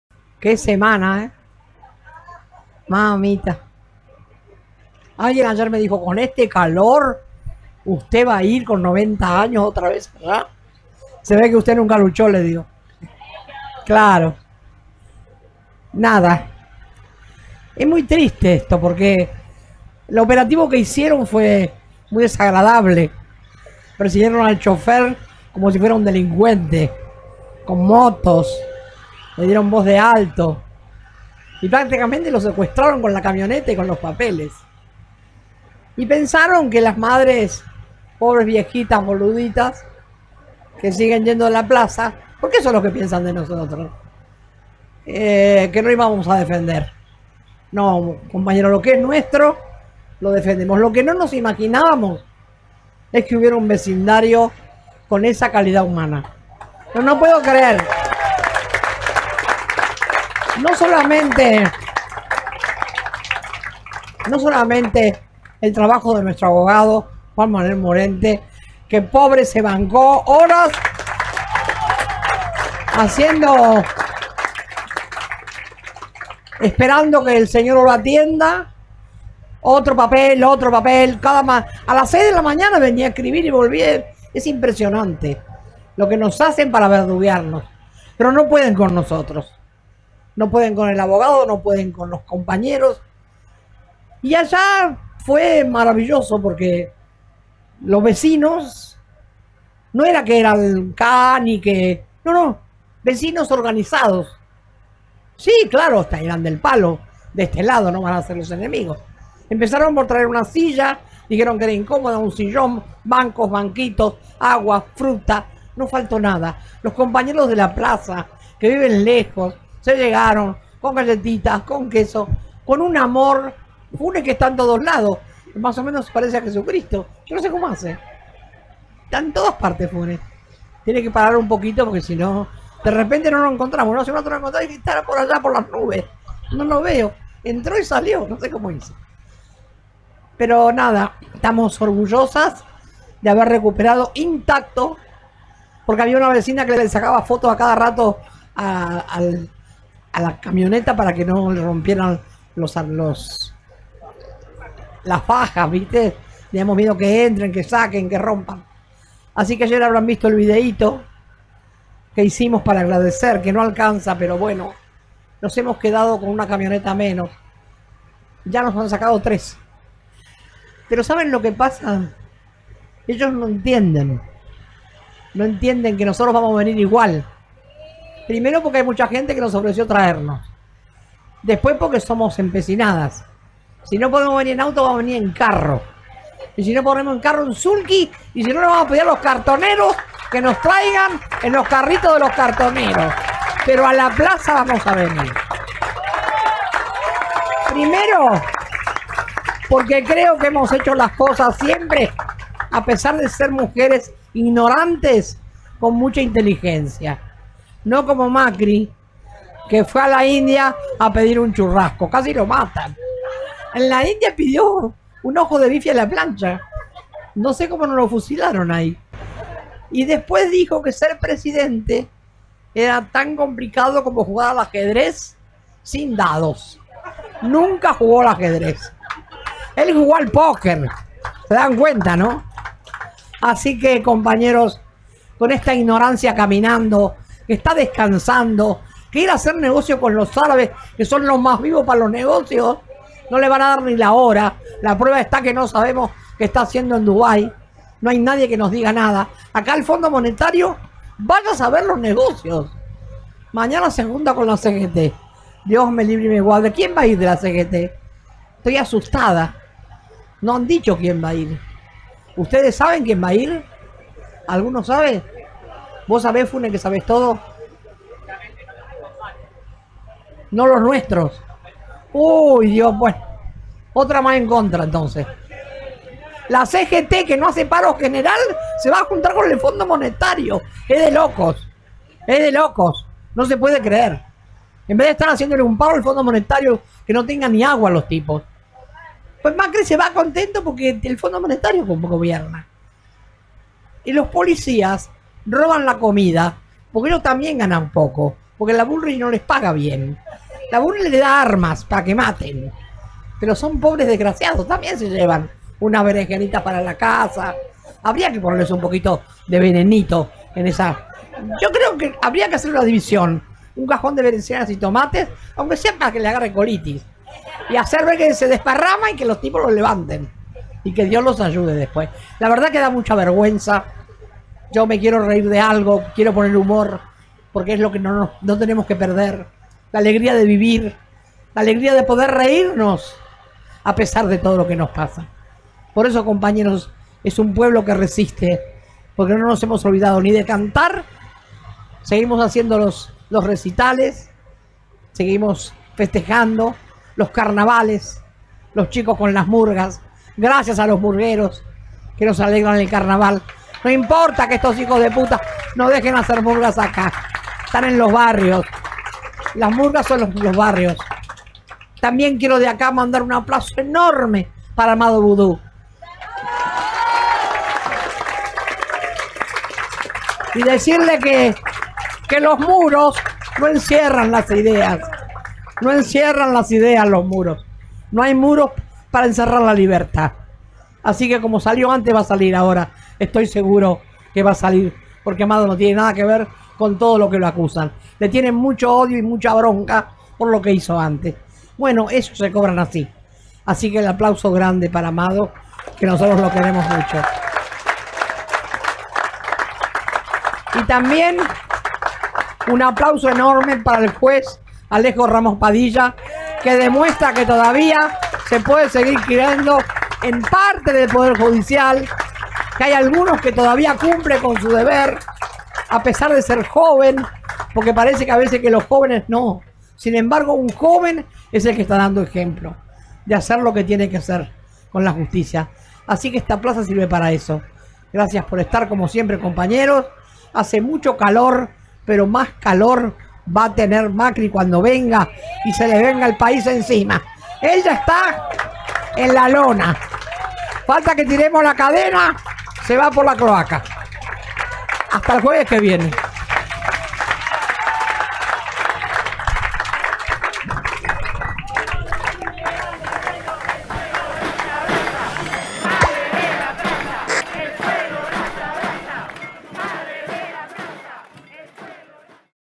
Marcha Nro 2132 21-2-19 Hebe de Bonafini
Marcha Nro 2132 de la Asociación Madres de Plaza de Mayo. Con las palabras de HebedeBonafini